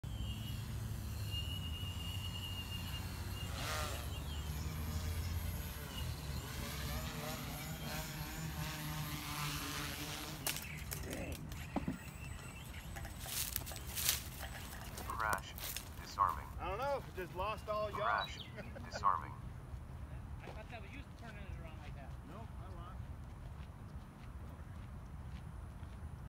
Tarot 650 sport uncontrolled yaw crash
maiden flight of my new build which resulted in an uncontrollable yaw spin all the way to the ground.  suspected a bad on-board compass so disabled it and flew again only with external compass but that too also resulted in a crash.

tarot 650 sport
tarot 15x5.5 carbon fiber props
px4 3.4.6
dragonlink v3
sunnysky 4112-320kv
hobbywing xrotor 40A esc
5000mah 6s X 1 = 2915g AUW